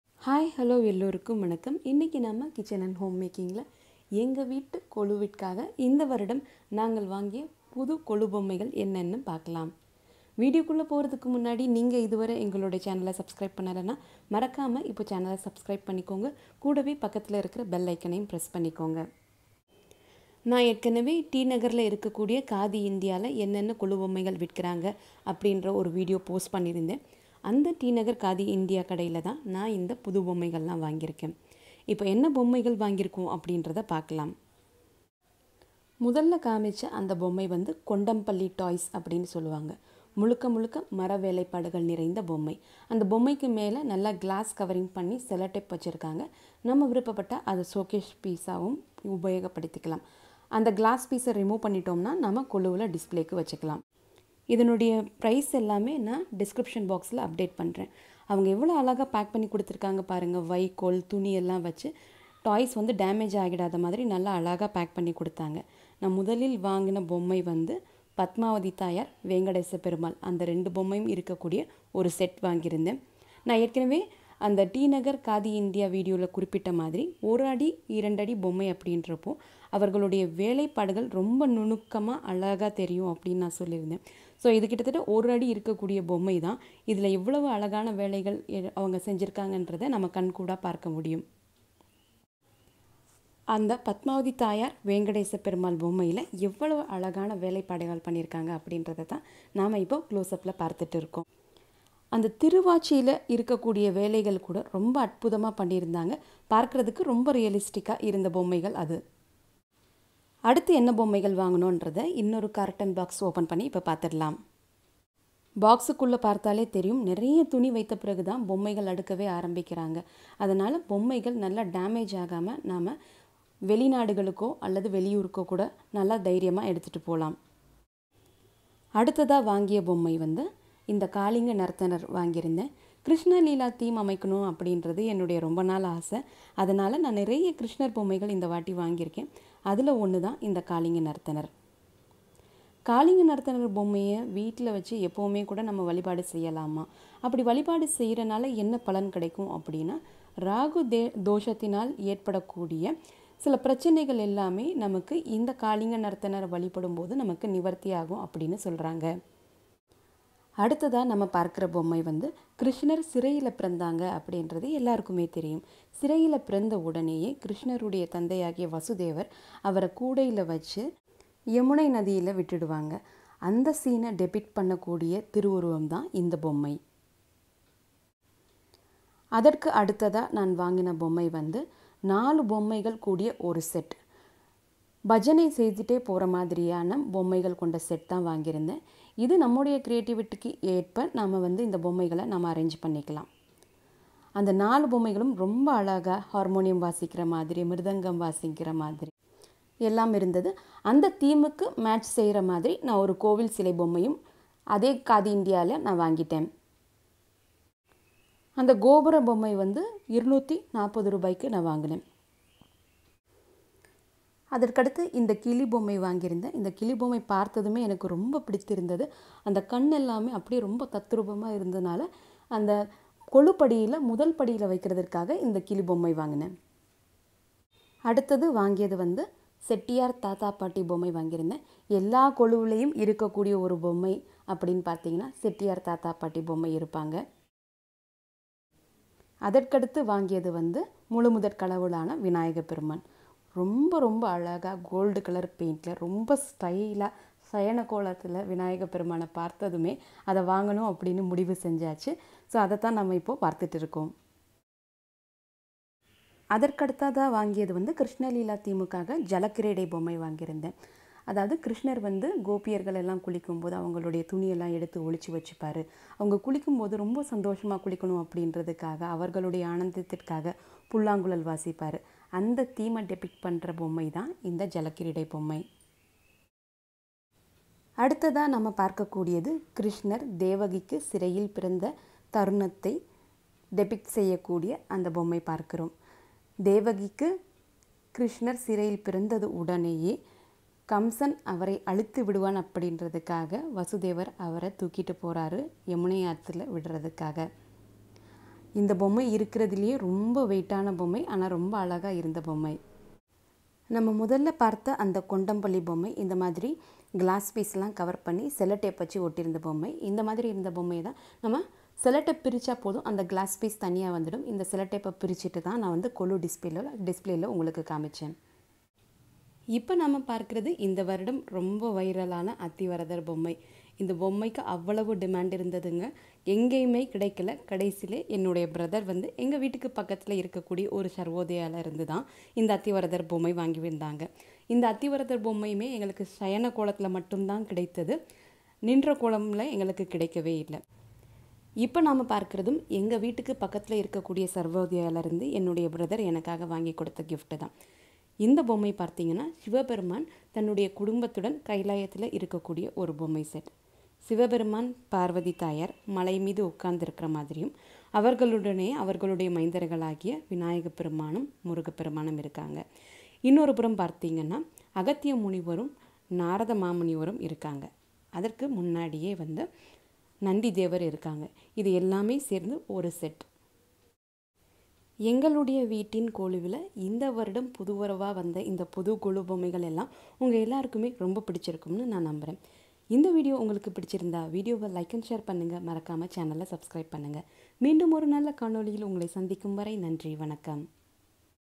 எங்கள் வீட்டு புது கொலு பொம்மைகள்/Shopping Haul - Kolu Dolls/ Golu Bommai
Dolls bought @ Khadi India in T. Nagar,  Chennai:
Perumal - Padmavathi Thaayar Set - Rs. 1300 ;
Vinayagar - Rs. 320 ( Am not sure of this rate. I think it ranged between  300 to 350)
Krishnar Birth in Jail - Rs. 800 ;
Vasudevar Carrying Krishna in Basket - Rs. ;
Kaalinga Narthanar - Rs. 600 ;
Krishnar Jalakreedai - Rs. 600;
Parrot - Rs. 40 ;
Chettiyar Thatha Paati - Rs. 350 ;
Bhajan Set - Rs. 300  ;
Temple / Gopuram - Rs. 240 ;

I don't know about the rate of Athivaradhar and sivakailayam set as I received it as gift.